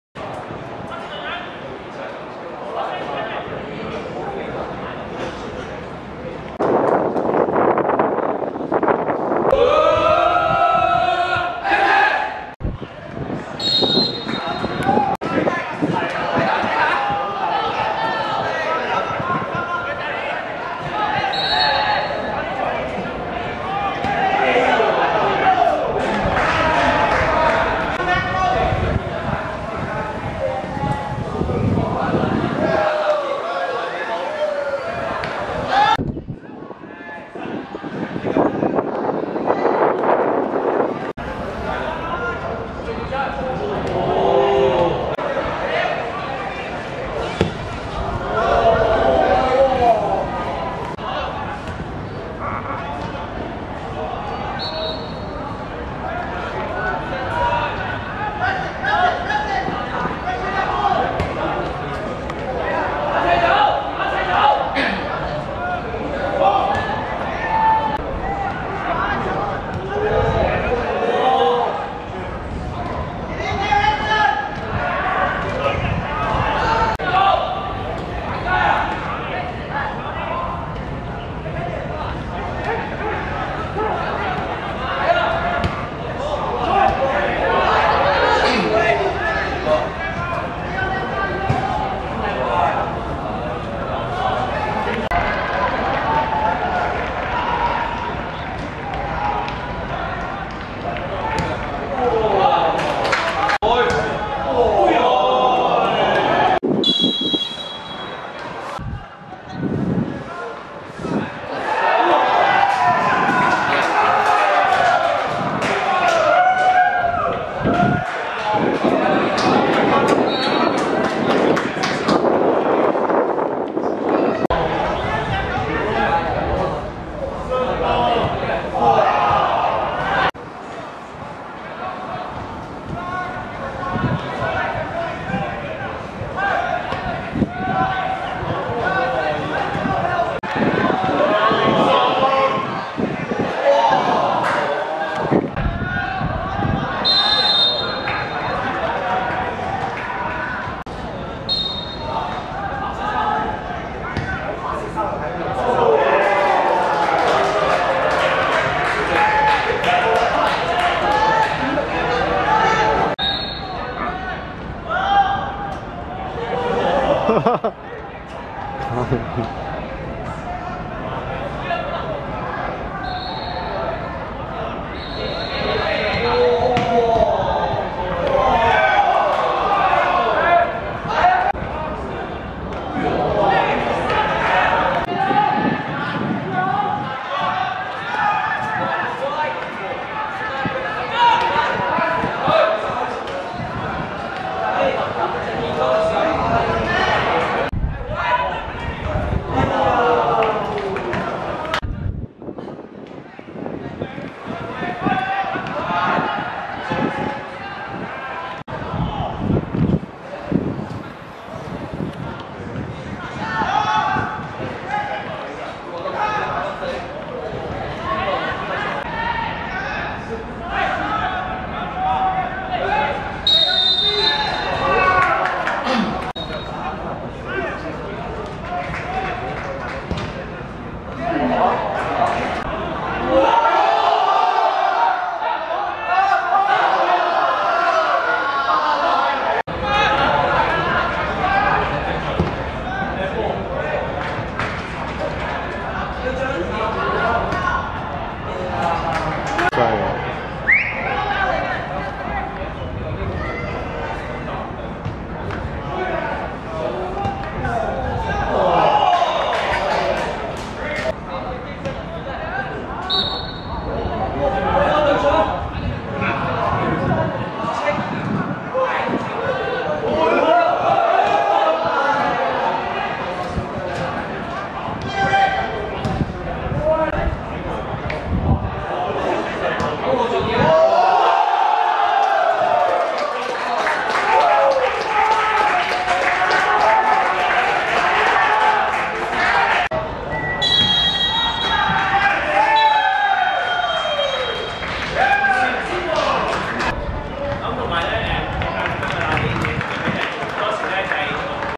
慈幼vs加拿大國際(2016.3.1.學界足球精英賽八強)精華
2016.3.1.蒲崗村仿真草塲 09:00 慈幼(黑) 1 : 2 加拿大國際(白)